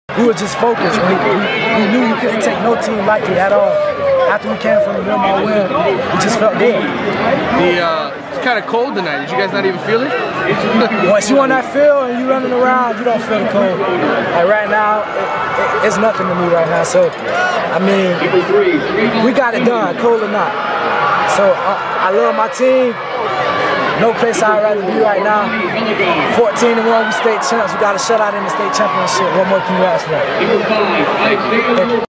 James Oliphant - St. Thomas Aquinas
St. Thomas Aquinas defensive back James Oliphant is interviewed after the Raiders' 31-0 rout of Kissimmee Osceola on Dec. 12, 2014.